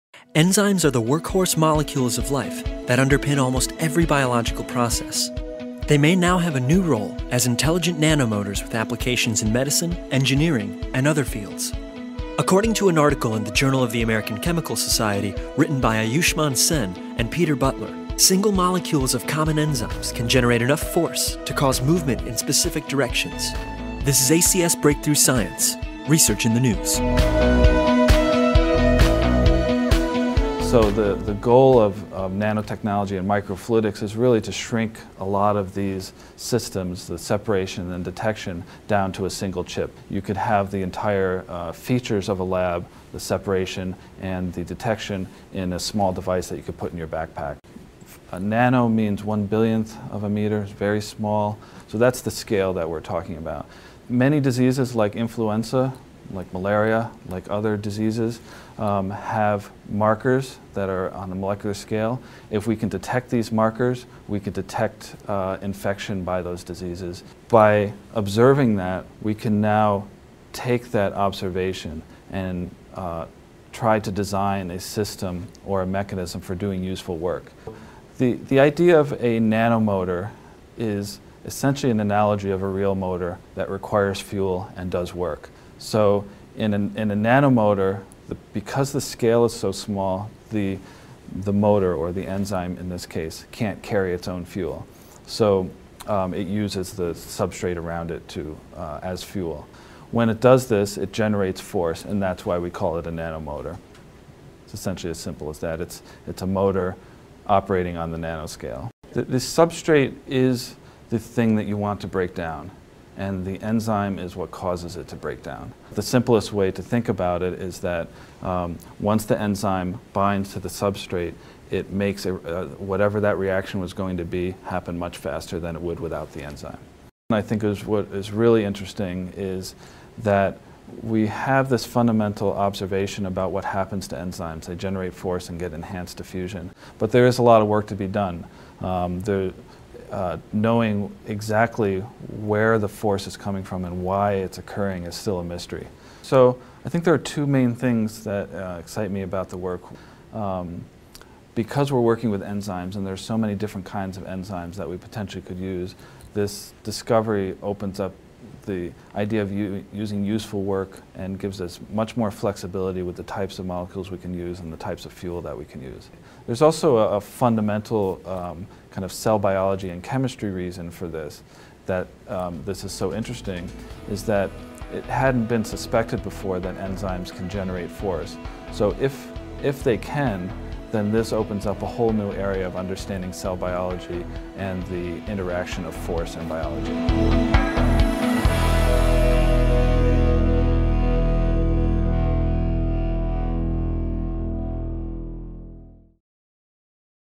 Enzyme Molecules as Nanomotors
In this video, the author discusses how enzymes could take on a new role as intelligent nanomotors with applications in medicine, engineering, and other fields.